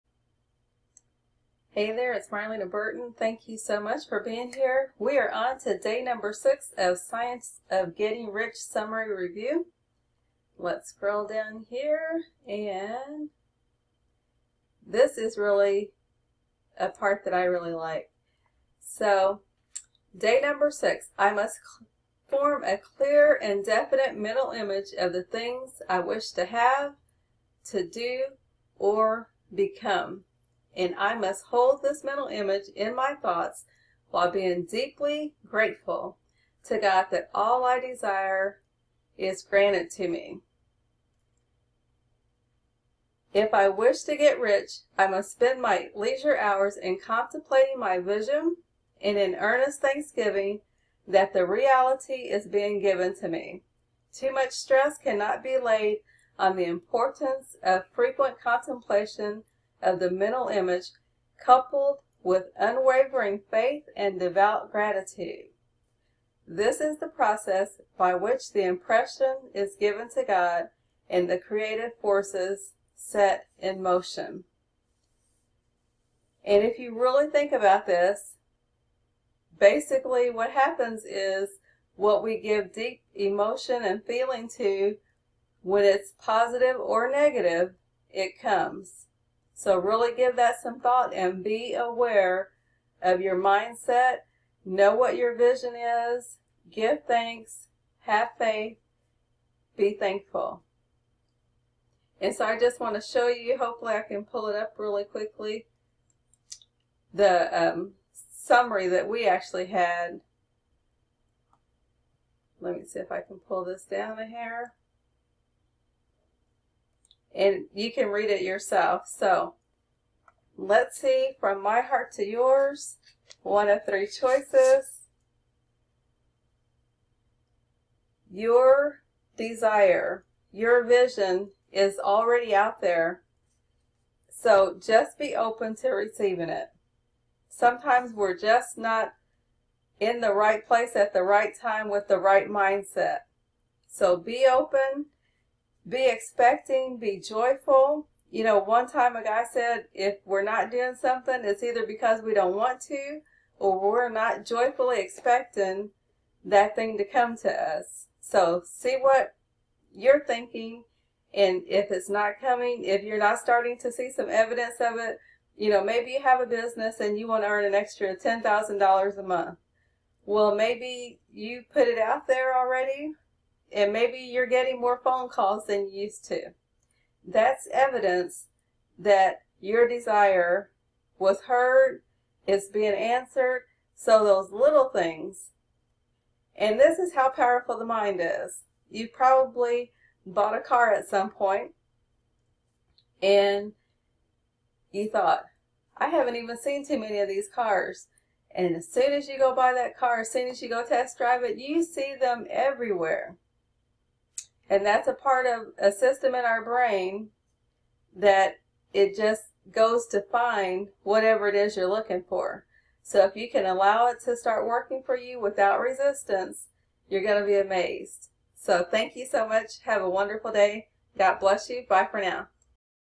SGR summary review day 6
Let's start exercising your receiving muscles and start reading the summary of the Science of getting rich summary daily.   We don't have to work as hard as we need to change our thinking.  Each day read the corresponding day with the day of the month, and focus on really getting understanding of the summary.  Read it over and over again.  So, the first of the month everyone will be reading day 1, the 10th of the month everyone will be reading the complete summary, which is great to read daily.  Day 1, 11, 21 start again on page 1 again,  so we will be reading it 3 times in a month.  Got it.  Let's go, get you some good results.  I Love my life, are you ready to wake up daily and say the same.  Health, money freedom, time freedom now is the time to live the lifestyle you deserve.